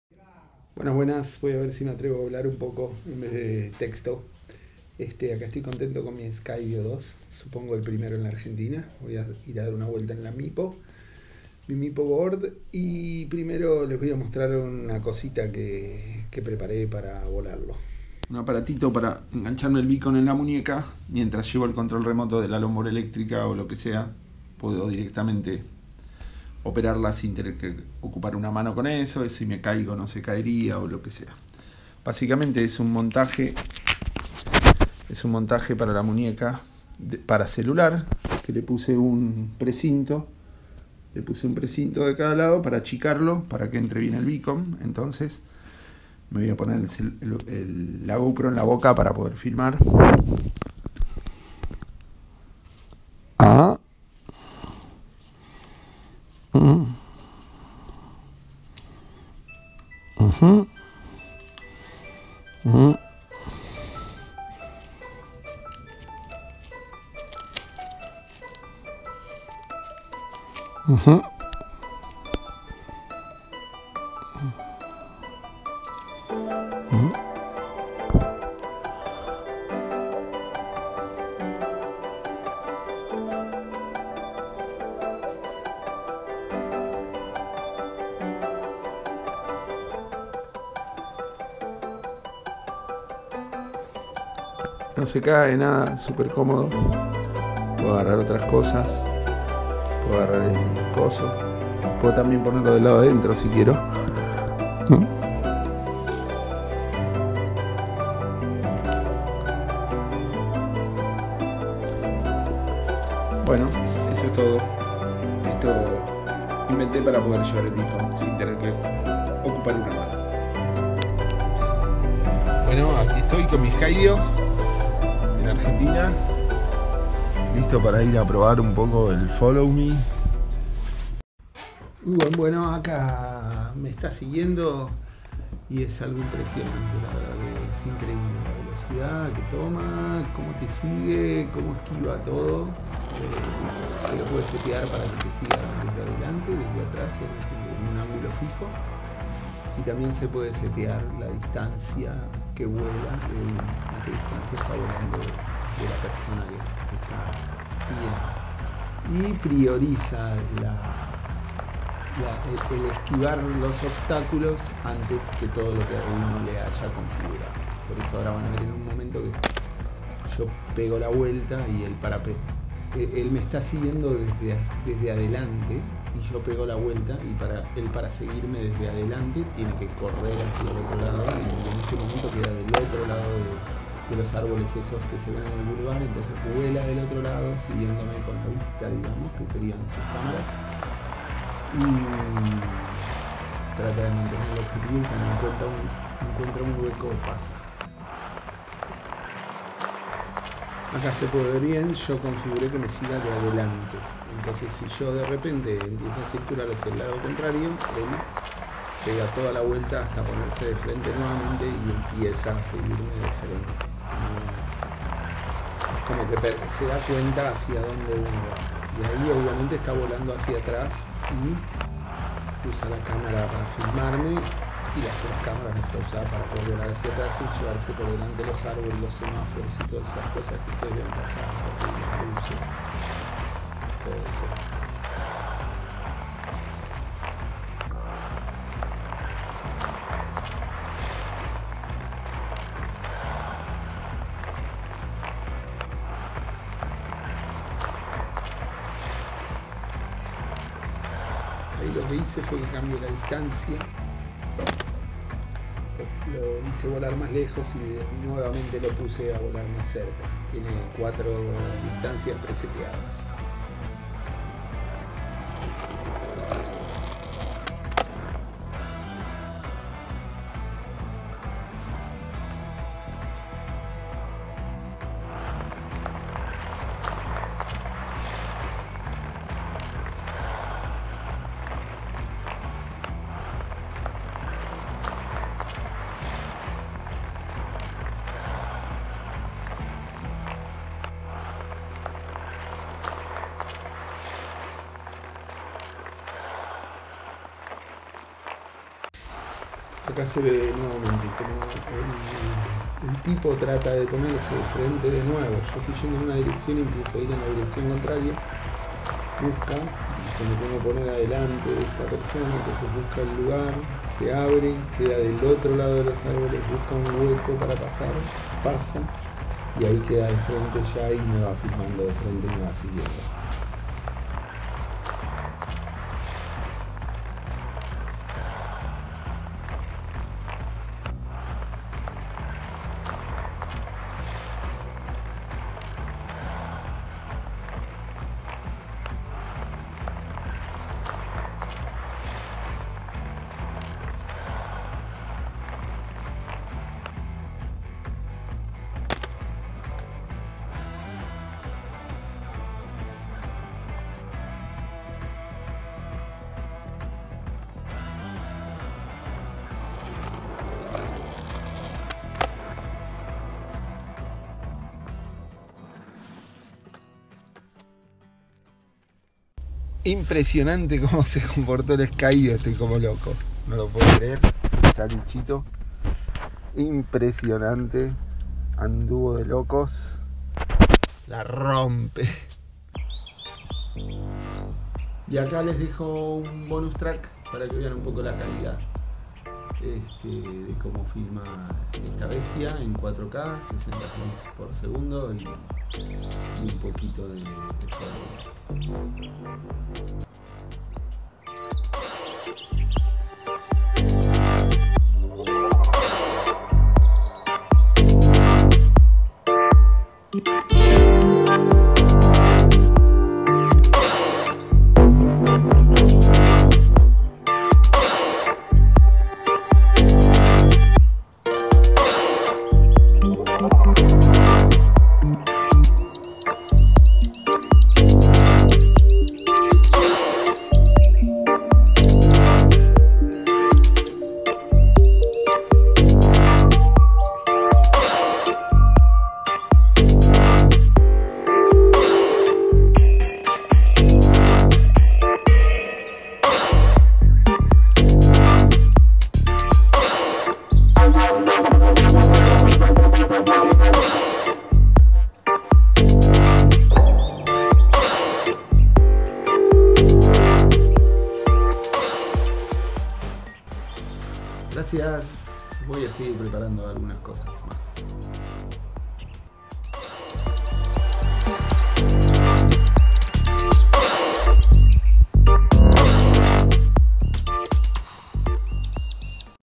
Primer Skydio 2 en Argentina - Brazalete para beacon - Testeando el seguimiento
Es mi primera vez volando el Skydio 2, este es el cinema kit con tres tipos de controladores, Móvil, Controlador y Beacon.La respuesta fué excelente en todos los aspectos, funciona muy bien en todas sus funciones y permite volar con total tranquilidad en cualquiera de sus modalidades.Realmente supera mis expectativas.

Volar hacia atrás con el drone fuera de linea de visión o en una perspectiva que no se puede calcular con los ojos era imposible para mí antes del skydio 2, el nivel de confianza que genera el vuelo autónomo es incomparable y la calidad de video es tremenda.

First Skydio 2 in Argentina - Creating a bracelet for the beacon - Testing the smart tracking - Observing the video quality

It is my first time flying the Skydio 2, this is the Cinema Kit with three types of controllers, Mobile, Controller, and Beacon.
The response was excellent in all aspects, it works very well in all its functions and allows you to fly with total peace of mind in any of its modalities. It really exceeds my expectations.

Flying backward with the drone out of the line of sight or in a perspective that cannot be calculated with the eyes was impossible for me before Skydio, the level of confidence generated by autonomous flight is unmatched and the video quality is tremendous